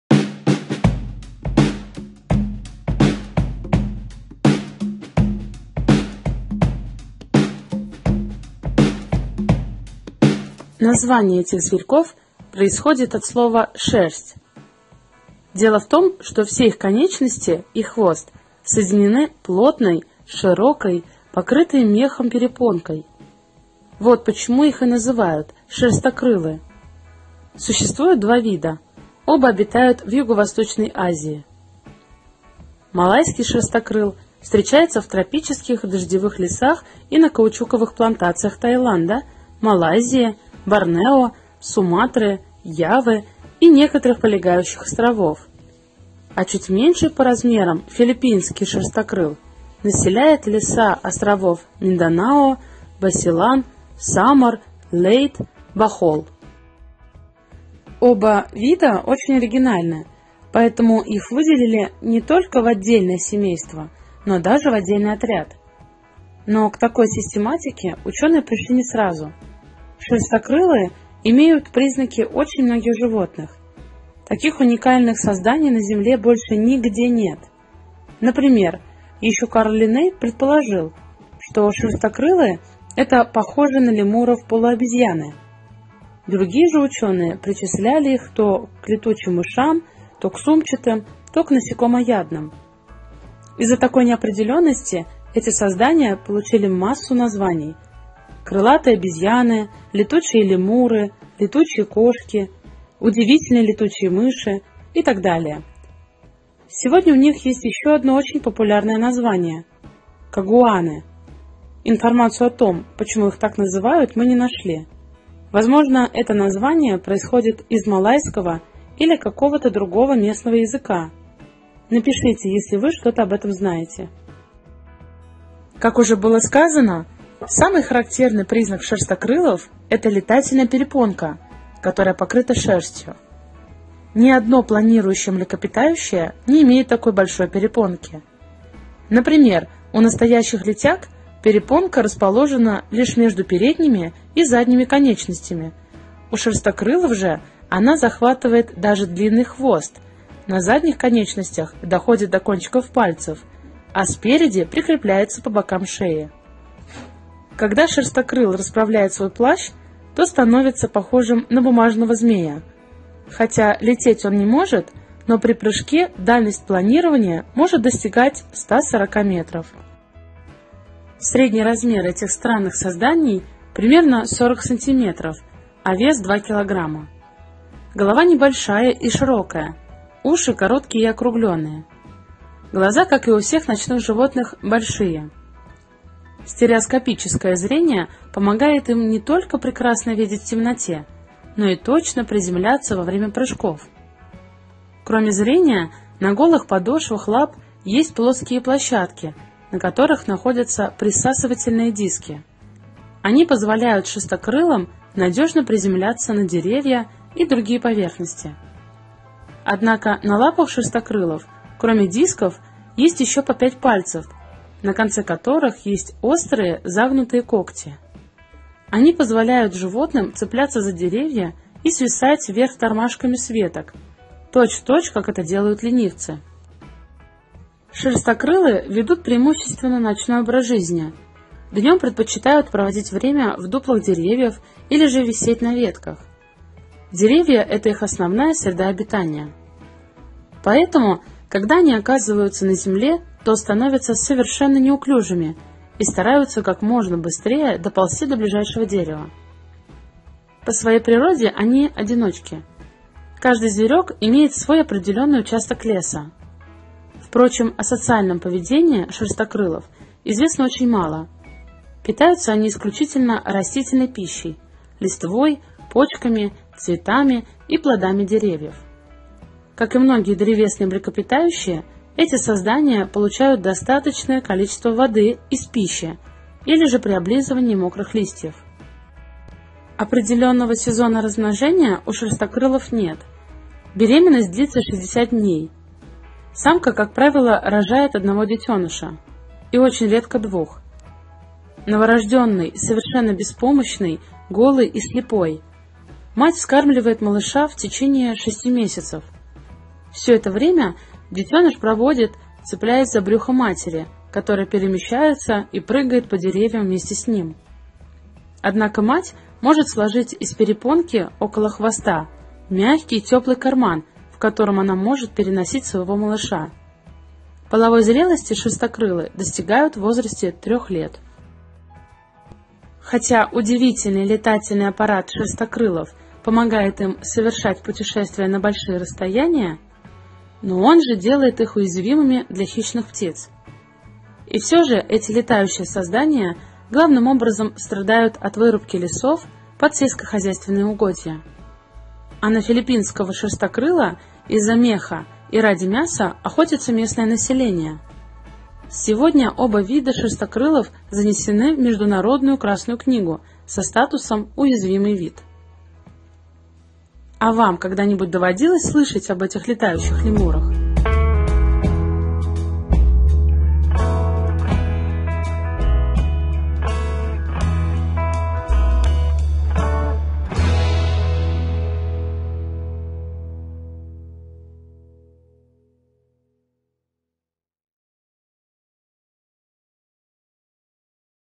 ШЕРСТОКРЫЛЫ - Летающие лемуры
Шерстокрылы имеют признаки очень многих животных. Таких уникальных созданий на Земле больше нигде нет!!! Например, ещё Карл Линней предположил, что шерстокрылы — это похожие на лемуров полуобезьяны. Другие же учёные причисляли их то летучими мышами, то к сумчатыми, то к насекомоядным. Из-за такой неопределённости эти создания получили массу названий: крылатые обезьяны, летучие лемуры, летучие кошки, удивительные летучие мыши и так далее. Сегодня у них есть ещё одно очень популярное название — кагуаны.